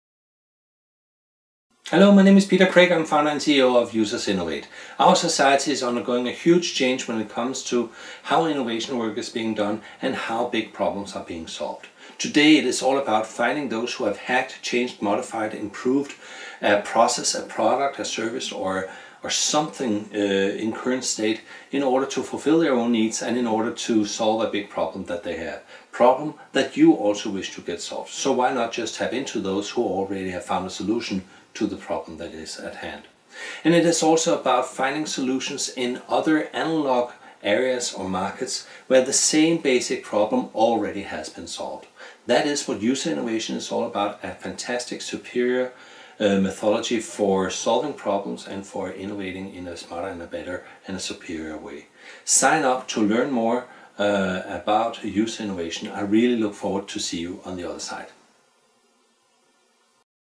Learn a new way to solve big problems & to innovate
Hallo, My name is Peter Kragh. I am founder and CEO of UsersInnovate.

Our society is undergoing a huge change in relation to how to innovate and how big problems are being solved.

Today, it is all about finding those who have hacked, changed, modified, improved or innovated on a product, a process, a service or something else in current state in order to fulfill their own needs and in order to solve a big problem that they have ... a problem that you also wish to get solved.

So why not tap into those who already have found a solution to the problem at hand.

And it is also about finding solutions in other, analogue areas or markets where the same basic problem already has been solved.

That is what user innovation is all about - innovating and solving problems in a new, smarter and superior way.

to learn more. I really look forward to see you on the other side.